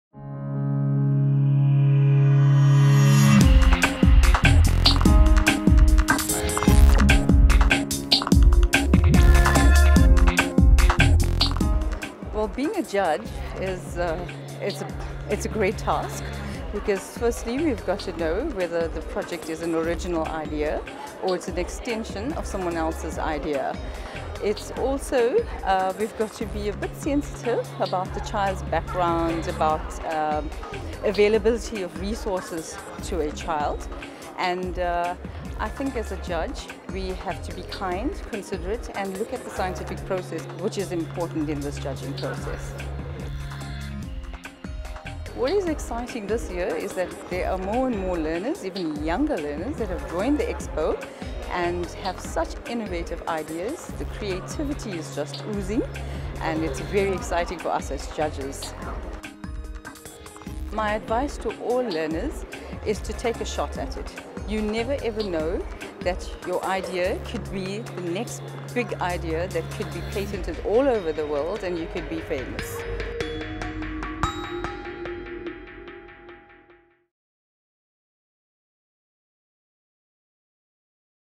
Chandhira Maniram on what it takes to participate in the EEYS
Adjudicator, Chandhira Maniram on what it takes to participate in the Eskom Expo for Young Scientists.